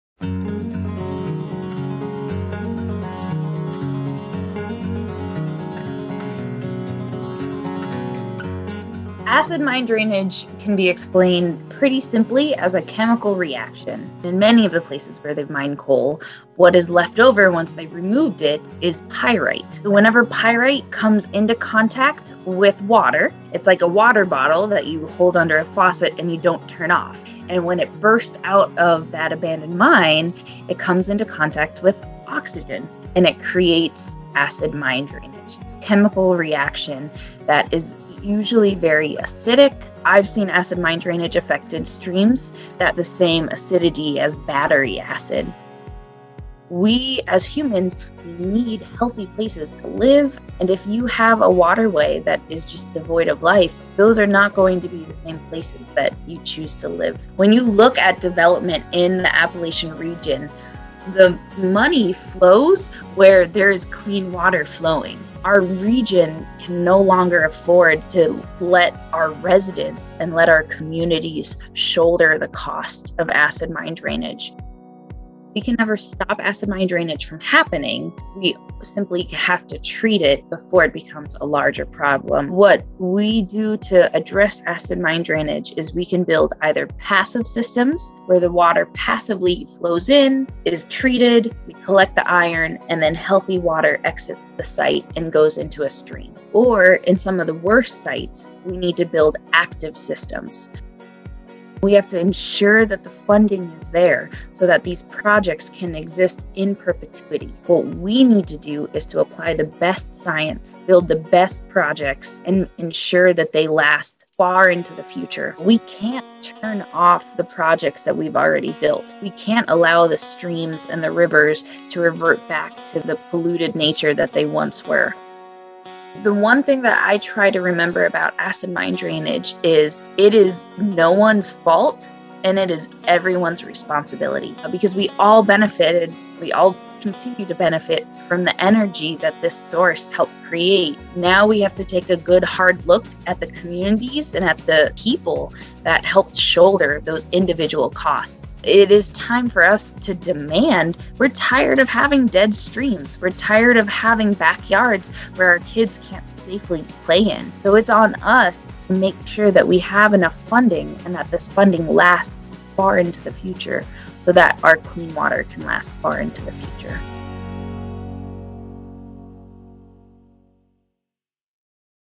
Confronting acid mine drainage: A conversation with Rural Action’s Marissa Lautzenheiser
The problems posed by abandoned mine lands come in many forms across places impacted by coal mining — and each contributes to making these communities less healthy, less safe, and less inviting for residents and businesses. Open mine portals can overflow, causing landslides. Dangerous impoundments rupture and cause toxic floods. Gob piles — massive heaps of coal refuse — seep into water sources. Flooded mines blow out, dumping hundreds of gallons of polluted water into streams and rivers. There are risks from hazardous gases, underground fires, clogged streams and more.

One of the more visible problems caused by abandoned mine operations is the acid mine drainage (AMD) that colors streams, creeks and rivers rust orange with dangerous pollution. These polluted bodies of water are an all-too-common roadside sight throughout coal communities, but they are more than just an eyesore. The ongoing threats posed by AMD threaten the health of people and wildlife and undermine development efforts. The Abandoned Mine Lands (AML) fund has helped clean up many sites impacted by acid mine drainage across the country, but an untold number still exist and clean-up projects already in operation must be operated indefinitely.

To share more about this urgent issue, advocates for abandoned mine land restoration talked to Marissa Lautzenheiser, who leads AMD clean-up efforts for Rural Action in Appalachian Ohio. In this conversation, Marissa lays out the basics of the AMD challenge, discusses the unique funding needs for AMD clean-up, and talks about some of the innovative work Rural Action is doing to tackle this problem.

Learn more about Rural Action's work to clean up acid mine drainage